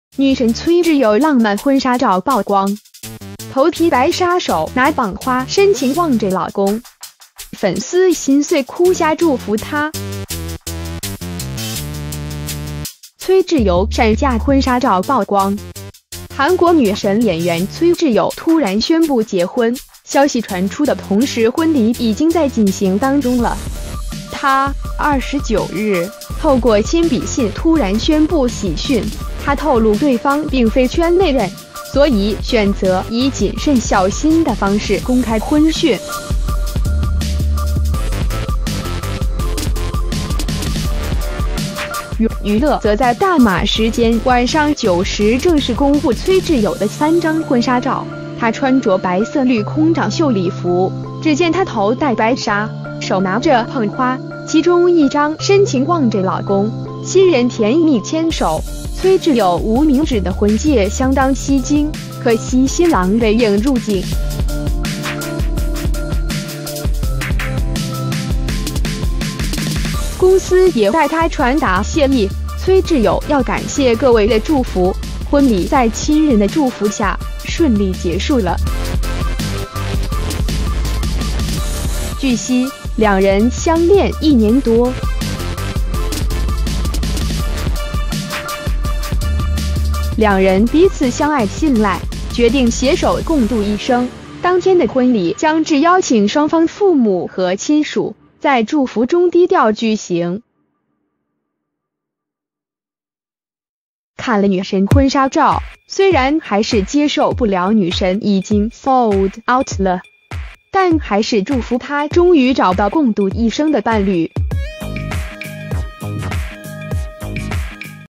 女神崔智友浪漫婚纱照曝光！头披白纱手拿棒花深情望着老公❤粉丝心碎哭瞎祝福她～
崔智友闪嫁婚纱照曝光 韩国女神演员崔智友突然宣布结婚，消息传出的同时婚礼已经在进行当中了！她（29日）透过亲笔信突然宣布喜讯，她透露对方并非圈内人，所以选择以谨慎小心的方式公开婚讯。